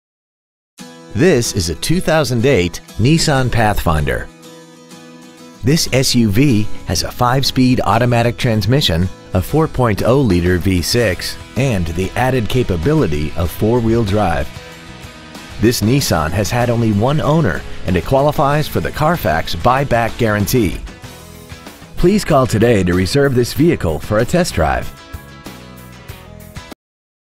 Pre-Owned 2008 Nissan Pathfinder Beaverdale PA 15921
Year : 2008
 Make : Nissan
 Model : Pathfinder
 Engine : V6 4.0 Liter
 Trans . : 5-Spd Automatic
 Exterior : Blue
 Miles : 30,948
 Interior : Black
 Stock : 605783